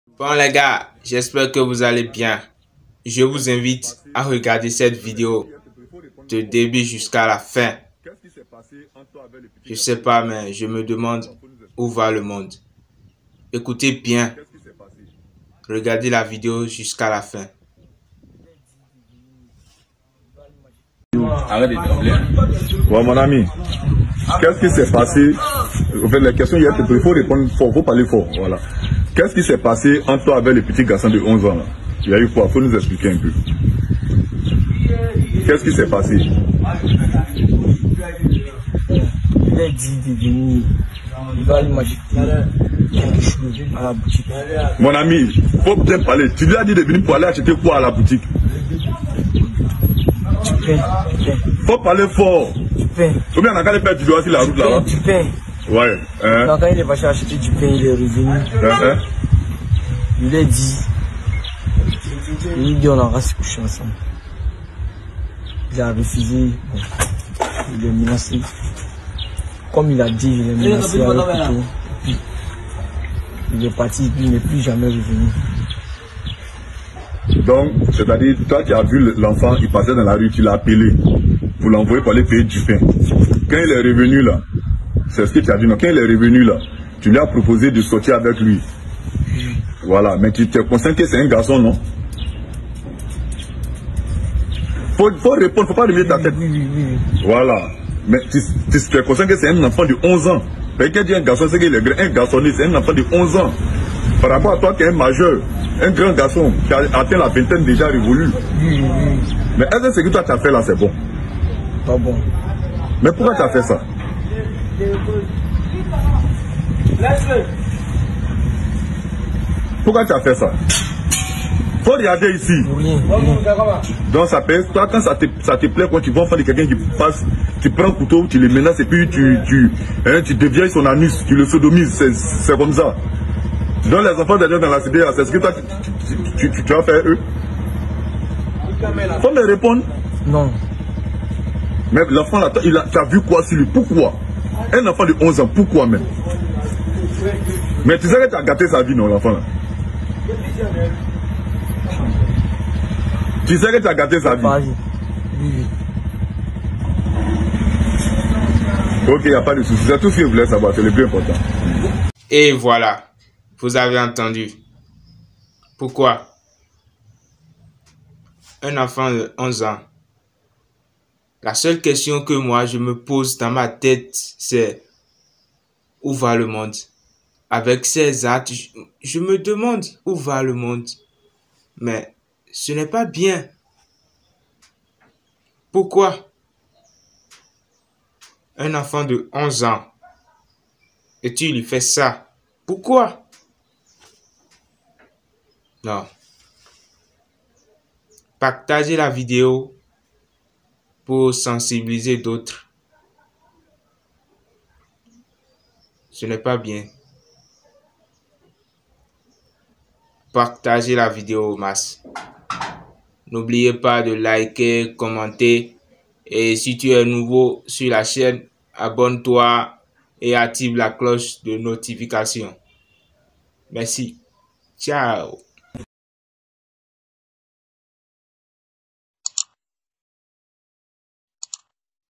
😭Urgent: cet jeune garçon mougou un petit garçon de 11 ans 😭😭
🚨🚨Urgent: Il sodomise un enfant de 11 ans 😭😭
Où va le monde 🙆🙆 pourquoi tout ça.Non à ces actes ❌❌❌
Partager la vidéo au max.